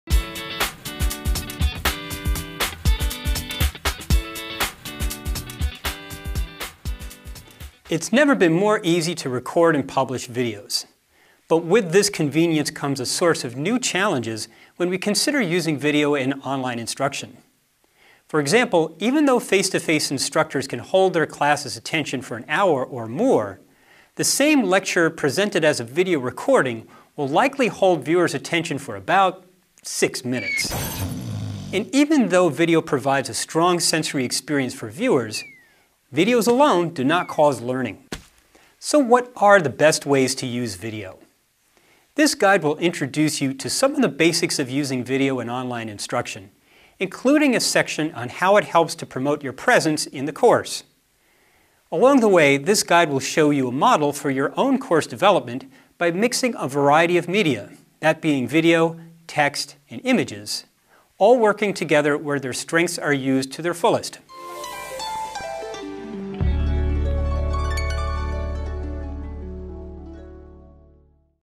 Using Video in Online Instruction - An Overview
This is an introduction to the major principles of using video in online instruction.